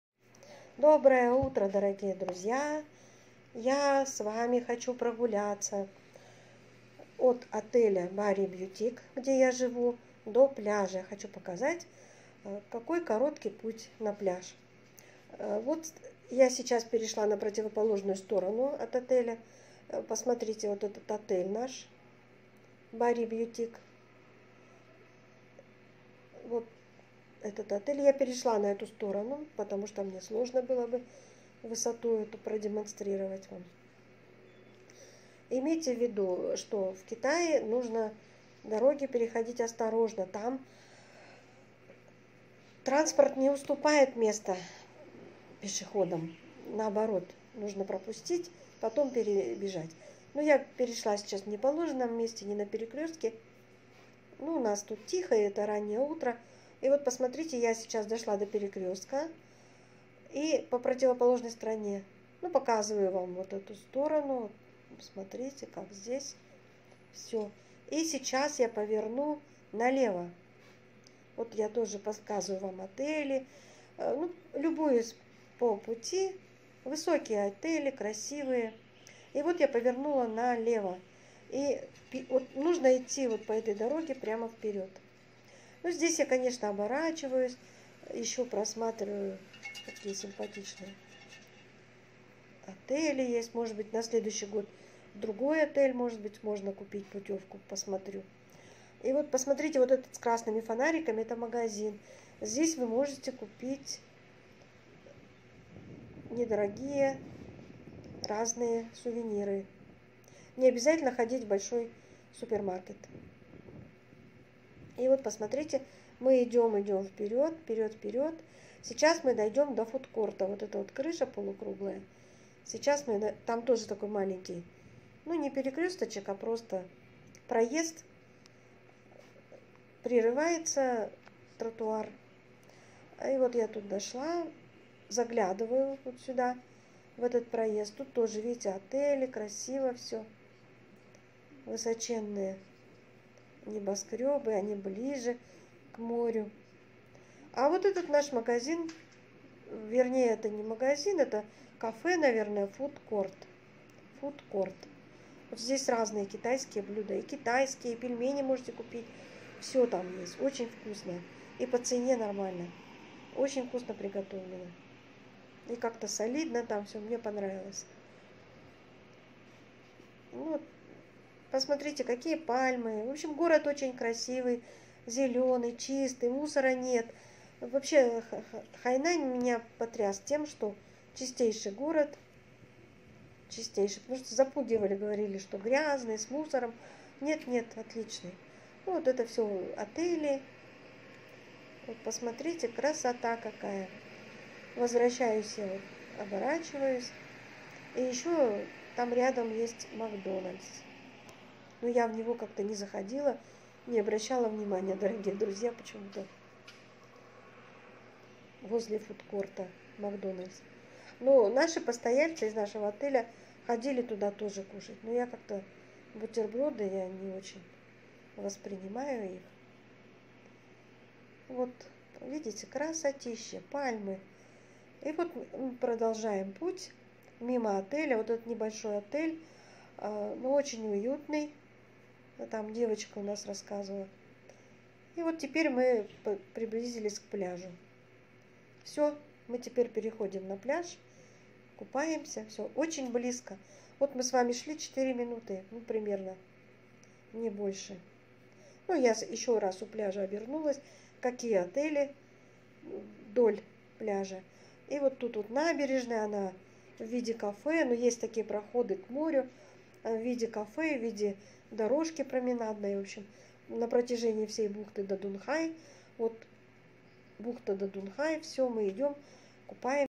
Barry Boutique 5* hotel. Дорога на пляж. Бухта Дадунхай. Отдых на Хайнане.
Barry Boutique 5* hotel.Дорога на пляж. Бухта Дадунхай. Хайнань.
Дорогие друзья приглашаю на раннюю утреннюю прогулку из  Barry Boutique 5* hotel на пляж. Мы с Вами находимся в районе бухты Дадунхай. Выхожу из отеля, перехожу дорогу, дохожу до перекрестка, поворачиваю налево, идем дальше прямо до набережной. Дорога занимает очень мало времени.